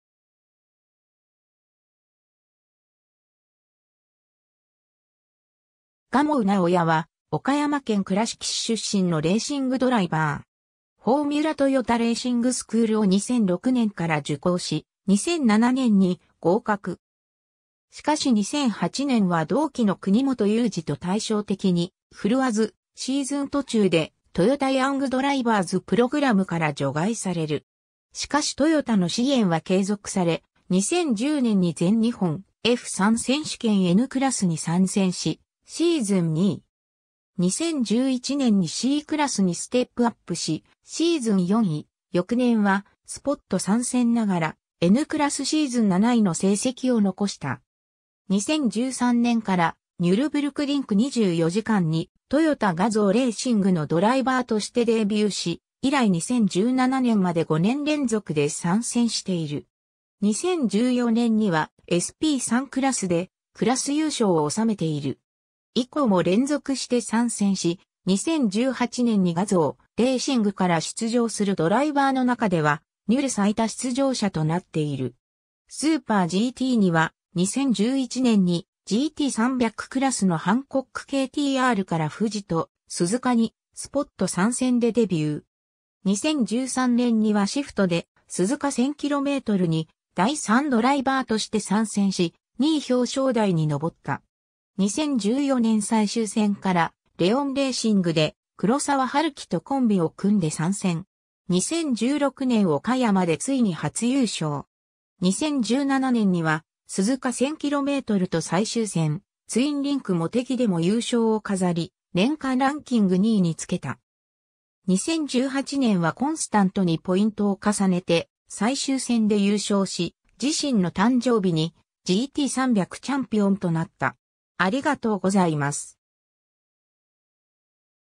蒲生尚弥
蒲生 尚弥（がもう なおや、1989年11月11日 - ）は、岡山県倉敷市出身のレーシングドライバー。
 フォーミュラトヨタレーシングスクール（FTRS）を2006年から受講し、2007年に合格。
しかし2008年は同期の国本雄資と対照的に振るわず、シーズン途中でトヨタ・ヤングドライバーズ・プログラム（TDP）から除外される。
しかしトヨタの支援は継続され、2010年に全日本F3選手権 Nクラスに参戦しシーズン2位。
2011年にCクラスにステップアップしシーズン4位、翌年はスポット参戦ながらNクラス シーズン7位の成績を残した。
 2013年からニュルブルクリンク24時間にTOYOTA GAZOO Racingのドライバーとしてデビューし、以来2017年まで5年連続で参戦している。
2014年にはSP3クラスでクラス優勝を収めている。
以降も連続して参戦し、2018年にGAZOO Racingから出場するドライバーの中ではニュル最多出場者となっている。
 SUPER GTには、2011年にGT300クラスのハンコックKTRから富士と鈴鹿にスポット参戦でデビュー。
2013年にはSHIFTで鈴鹿1000kmに第3ドライバーとして参戦し、2位表彰台に登った。
 2014年最終戦からLEON RACINGで黒澤治樹とコンビを組んで参戦。
2016年岡山でついに初優勝。
2017年には鈴鹿1000kmと最終戦ツインリンクもてぎでも優勝を飾り、年間ランキング2位につけた。
2018年はコンスタントにポイントを重ねて最終戦で優勝し、自身の誕生日にGT300チャンピオンとなった。